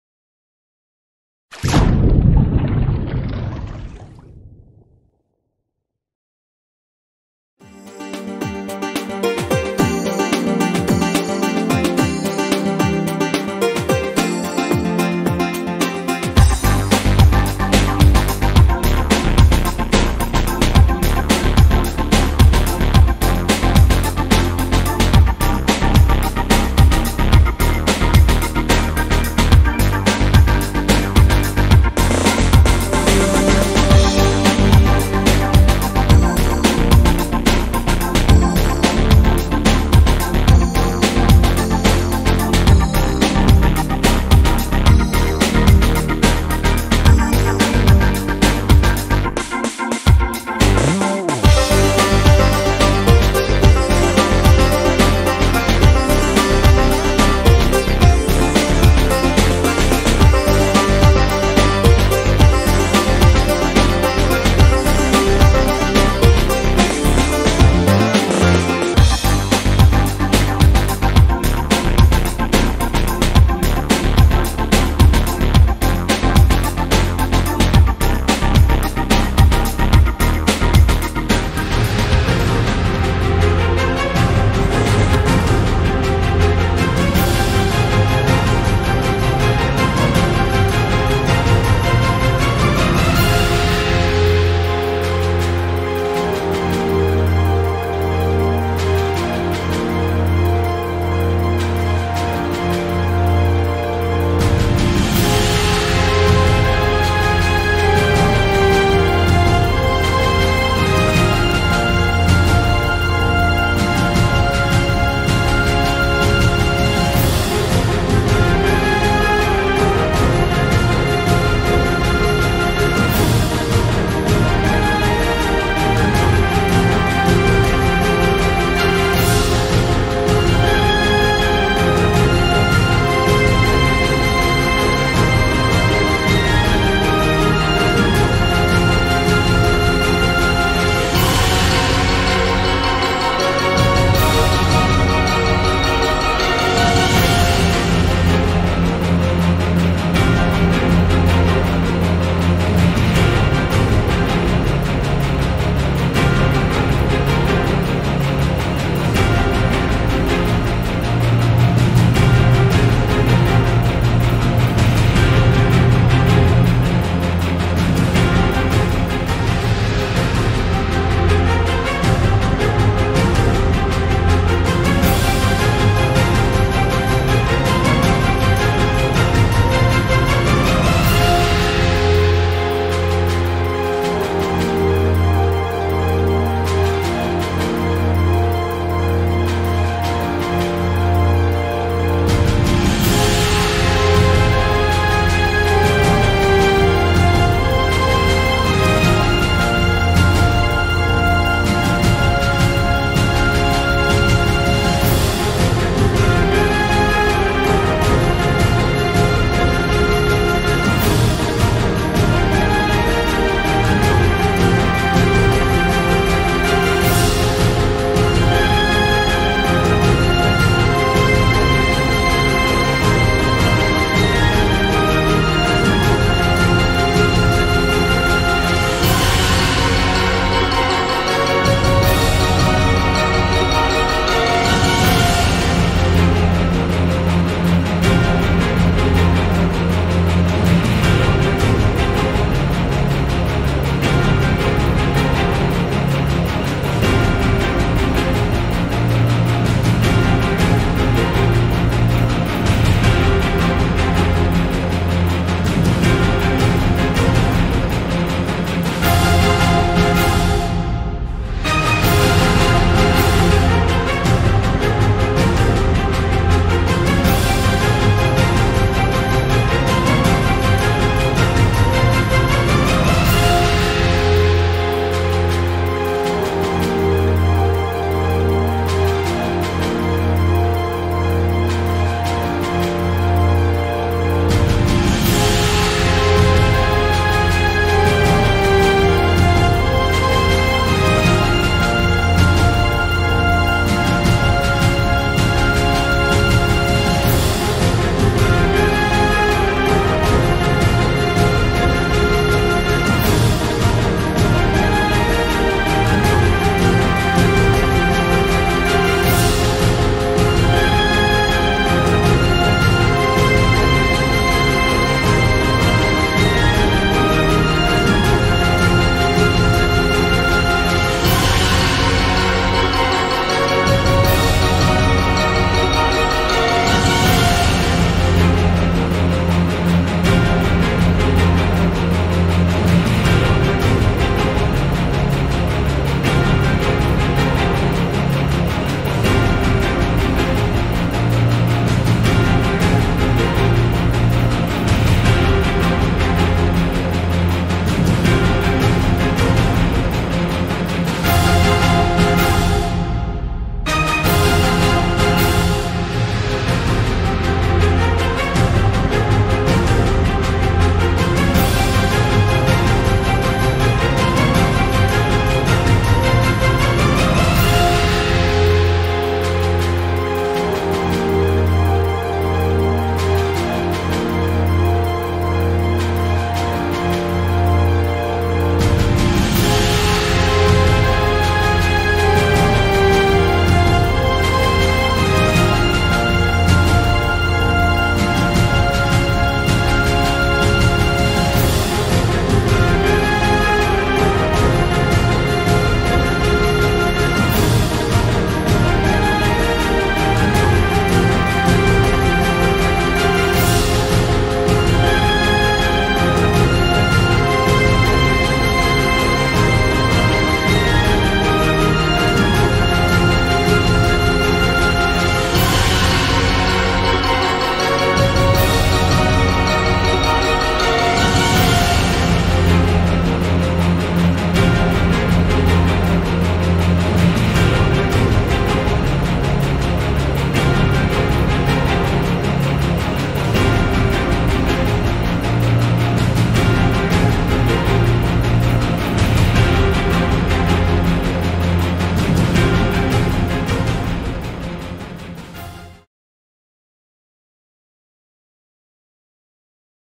RESUMEN FOTOSUB 2019
Les presentamos un resumen del Evento de Fotografía Subacuática que tuvo lugar en el Centro Internacional de Buceo Faro Luna, Cienfuegos, Cuba, 2019. En el 2021 volveremos a las aguas a capturar las imágenes de este bello lugar.